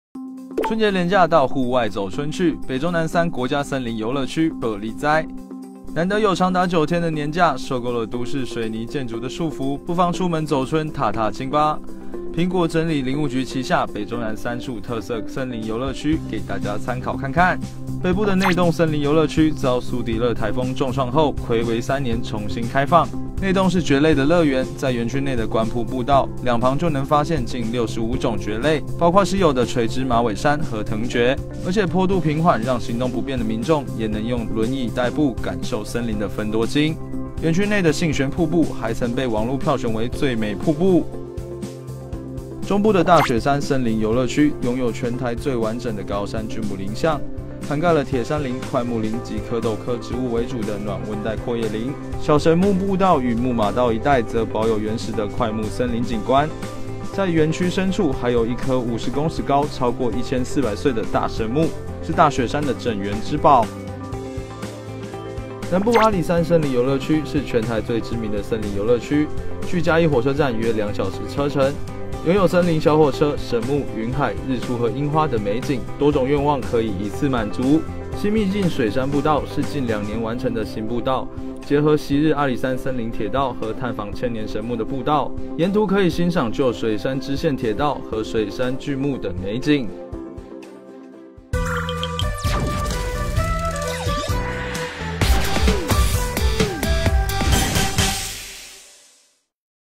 過年踏青 這3處超美森林遊樂區可別漏勾 | 台灣蘋果日報
受夠都市水泥建築的束縛，不妨出遊踏踏青，享受森林自然芬多精。《蘋果》整理林務局旗下3處超吸睛國家森林遊樂區，北部內洞睽違3年重新開放，南部阿里山新秘境「水山步道」是近2年新完成步道，另中部大雪山則擁有全台最完整的高山巨木林相。